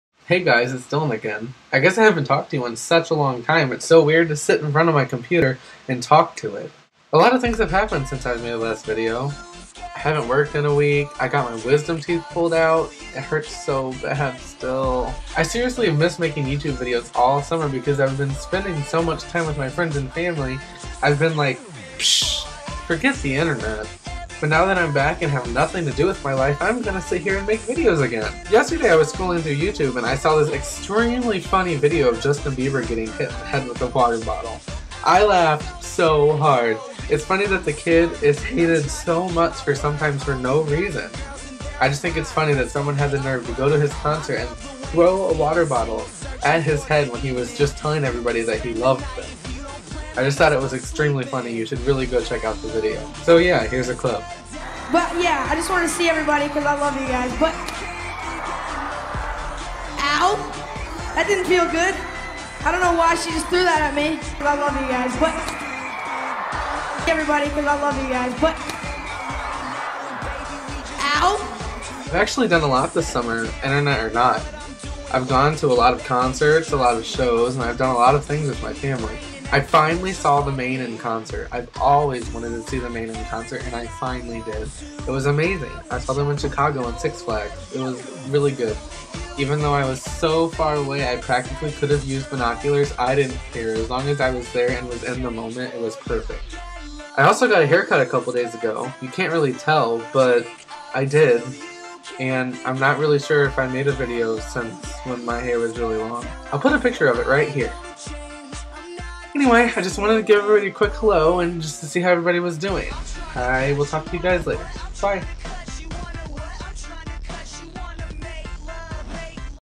Justin Bieber Gets Hit With A Water Bottle?
Just an update, mainly. BUT.
It is extremely funny that he got hit with a water bottle. Poor kid though, he really didn't do anything to anybody.
Hate his music, but I kinda feel bad.